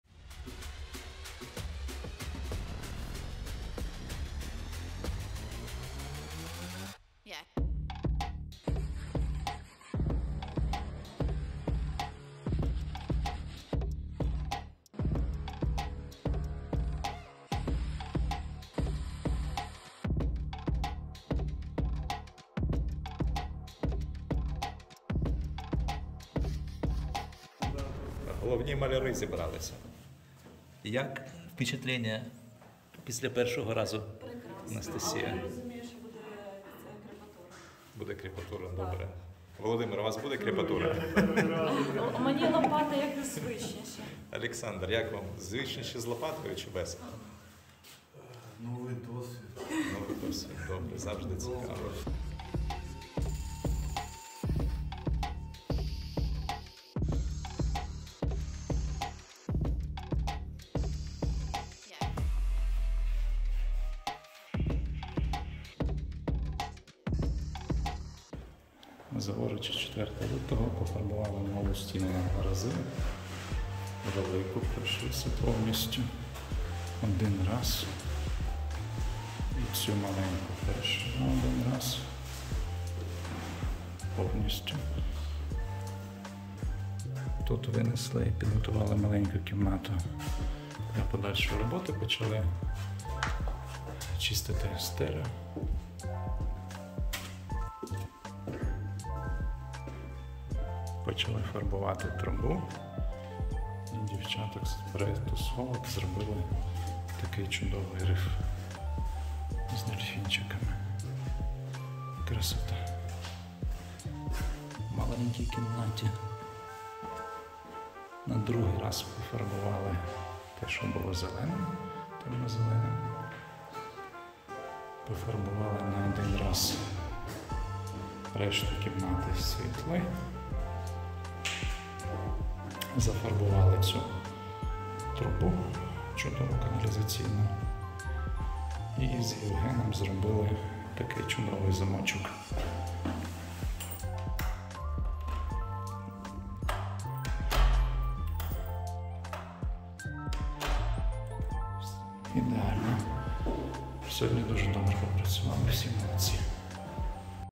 B50: progress in repairing the children's bomb shelter in Zavorychi village by our volunteers
The transformation of the kids' bomb shelter in Zavorychi Gymnasium (Brovary direction) is underway.  Our volunteers are transforming a gloomy basement into a convenient and comfortable shelter for preschoolers and primary school students. Our task is to repair and decorate 4 rooms of the shelter with drawings and arrange technical areas.
The volunteers of the Reconstruction project have already finished 3 rooms!

Please join us in helping by donating for paint and materials or spreading the word about volunteering and the needs of Ukraine.
Thanks for the video editing @on_altinci